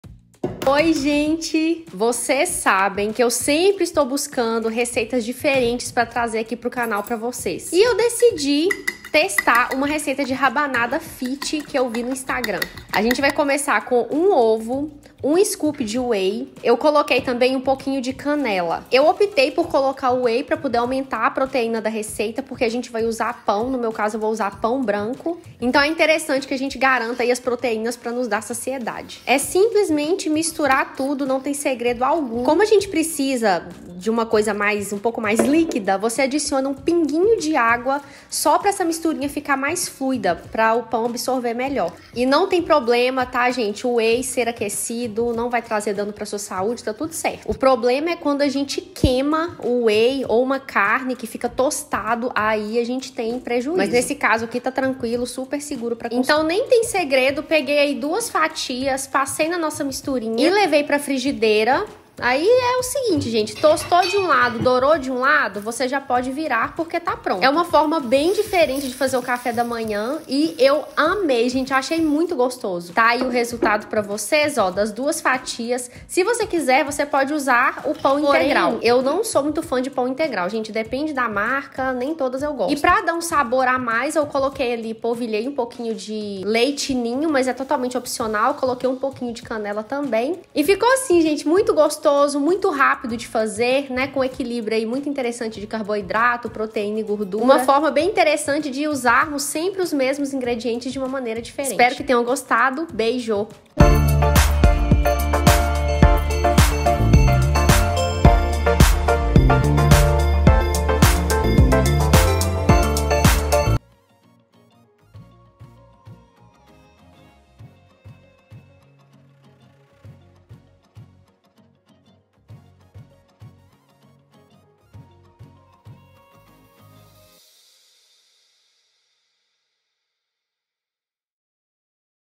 Cansada de dieta? Veja esta rabanada proteica, rápida e fácil de fazer
Ingredientes
1 ovo
15g de Whey
Água
Fatias de pão
Canela a gosto
Leite em pó a gosto